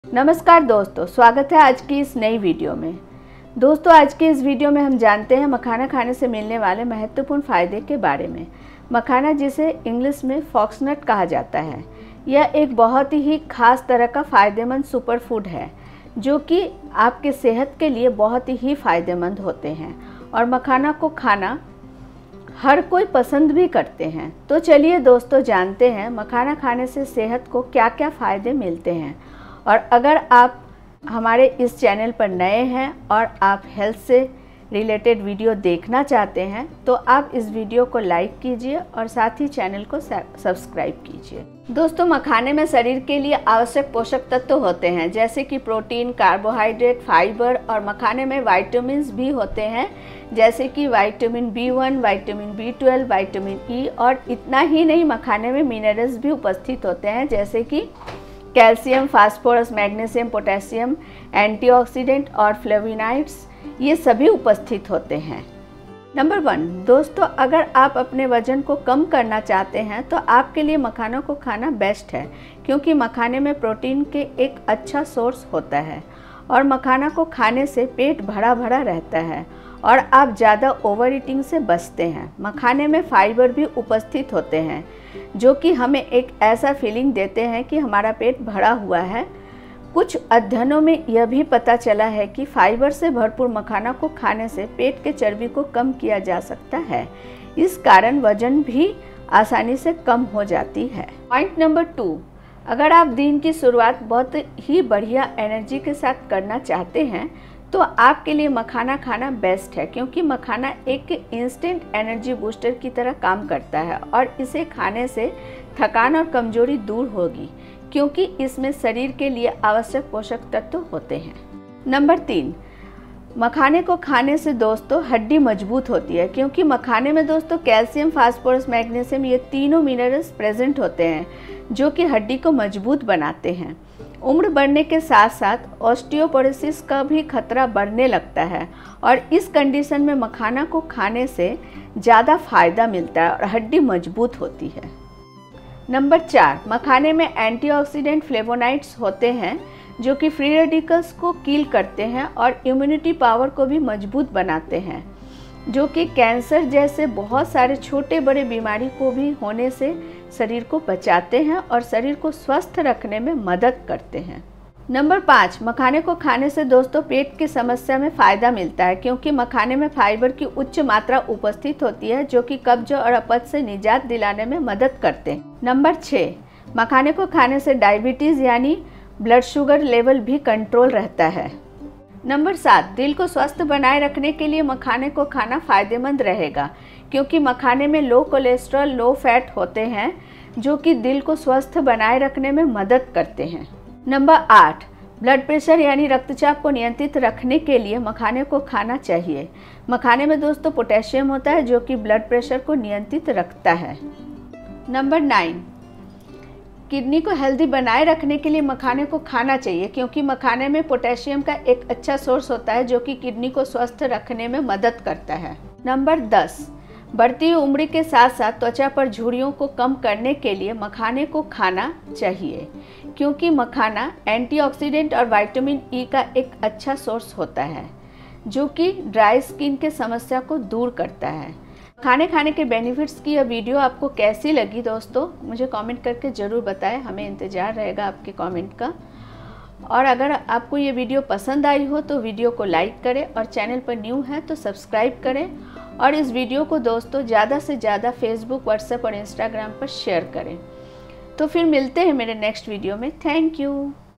मखाना खाने के ये 10 जबरदस्त फायदे आपको हैरान कर सकती है ! Makhana fayde|| Fox nuts benefits
ka khane fayde || Fox nuts benefits

मखाना खाने के जबरदस्त फायदे

मखाना खाने से मोटापा कम होता है

मखाना खाने से दिल स्वस्थ रहता है

मखाना खाने से किडनी स्वस्थ रहता है

मखाना खाने से त्वचा पर चमक रहता है

मखाना खाने से रक्त चाप ठीक रहता है

मखाना खाने से शरीर को ऊर्जा मिलती है

 मखाना  खाने से हडी मजबूत होती है